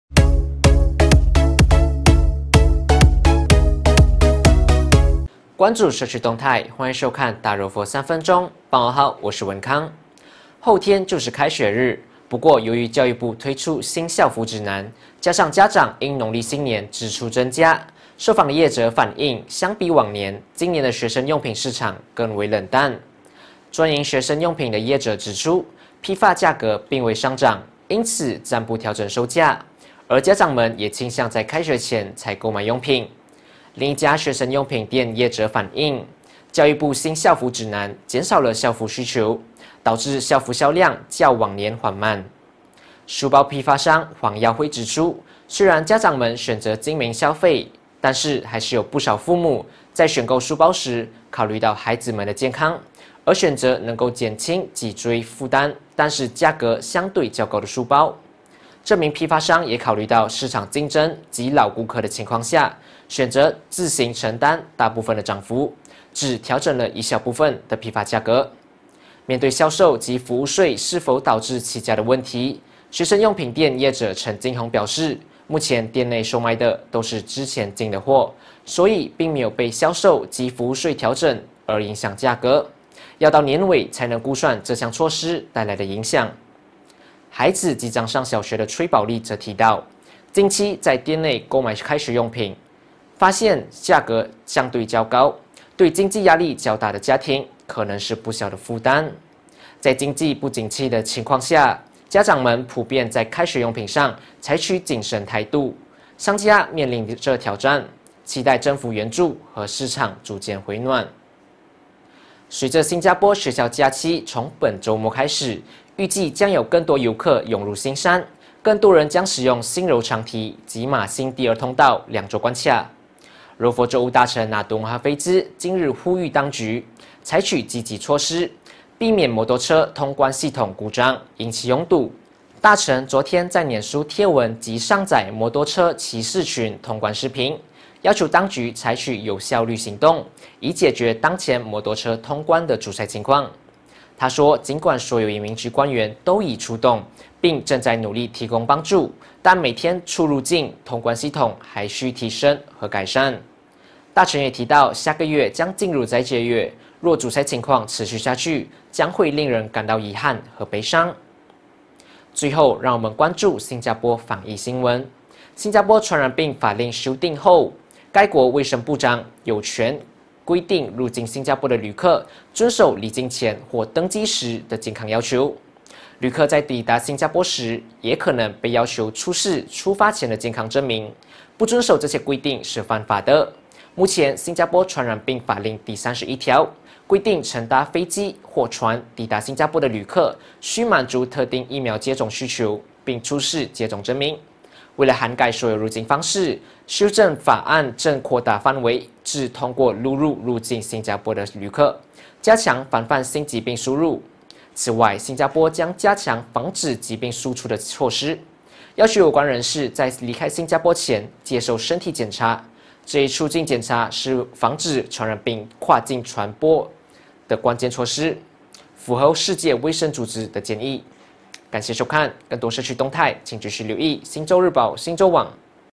大柔佛三分钟 | 新校服指南及家长荷包“未回血”等因素影响 开学用品市场较往年淡静
今日焦点：

🔹新校服指南及家长荷包“未回血”等因素影响 开学用品市场较往年淡静

🔹大臣：新加坡学校假期将至吁当局确保通关顺畅

🔹加强防止疾病输入或输出 狮城卫长可规定旅客出示健康证明